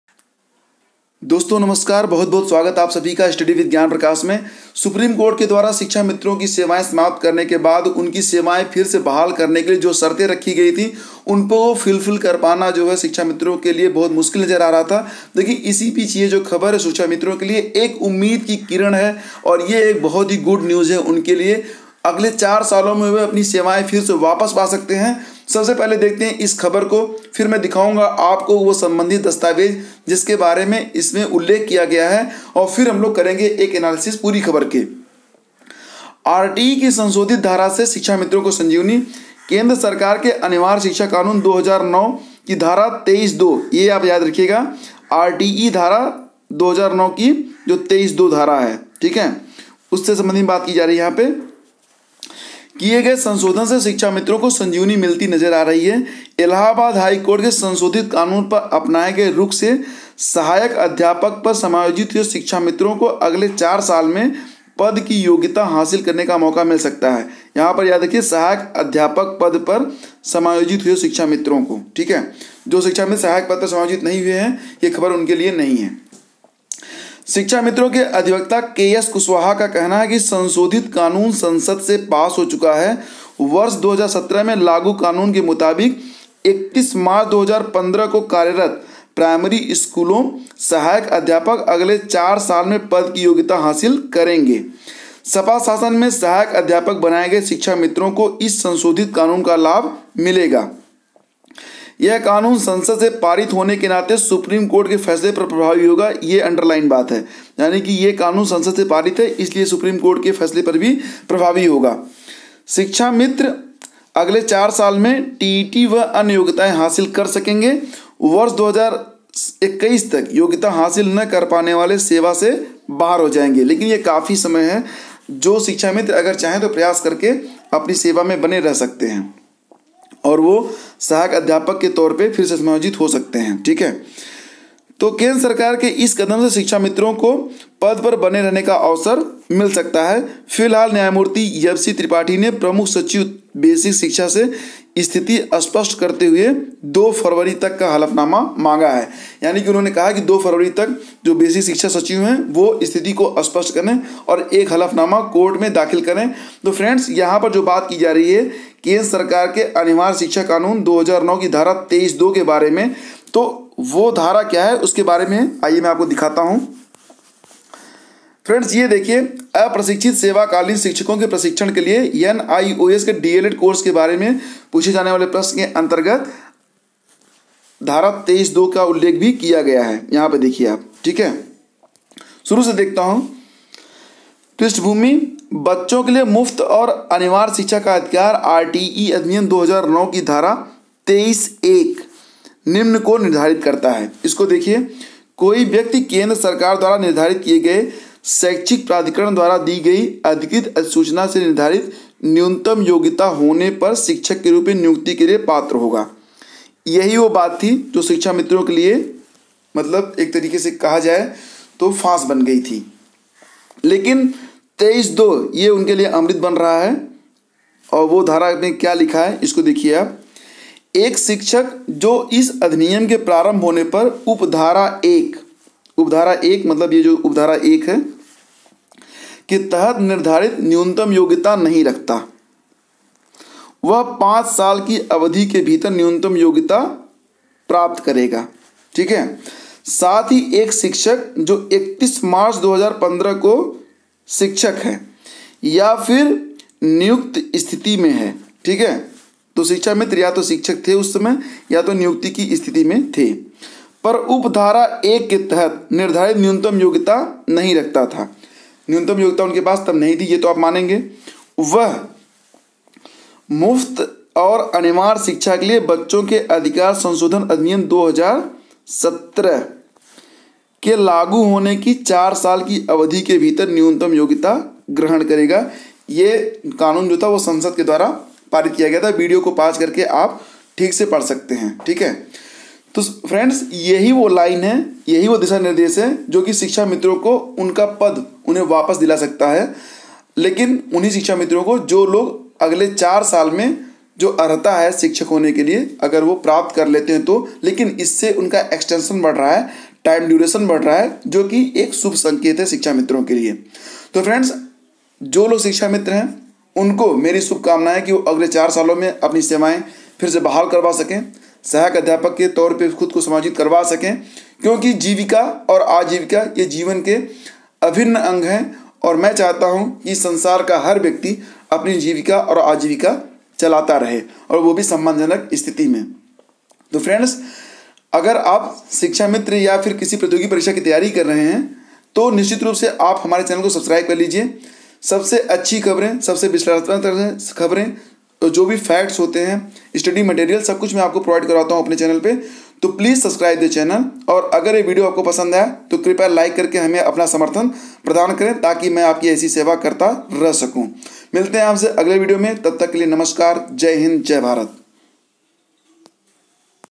शिक्षामित्र सहायक अध्यापक के पद होंगे फिर से बहाल अगर..
RTE ACT में हुए नवीन संशोधनों से शिक्षामित्रों को उनका सहायक अध्यापक का पद वापस मिलेगा। बस उन्हें कुछ फॉर्मेलिटीज को पूरा करना होगा।
शिक्षामित्रों के लिए जरूरी खबर।

हमारी वेबसाइट

Hamse judiye

Join our telegram channal